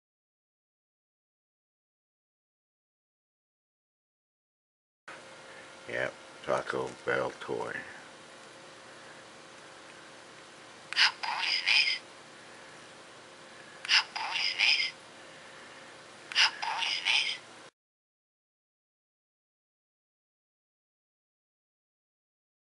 McD feat Taco Bell
About a hunnert years ago, Taco Bell used a talking chiuaua to peddle their wares. They sold these little talking versions that said things like "Yo quiero Taco Bell"  and this one, that McDs advertising agents apparently thought everyone who would remember it had died long ago.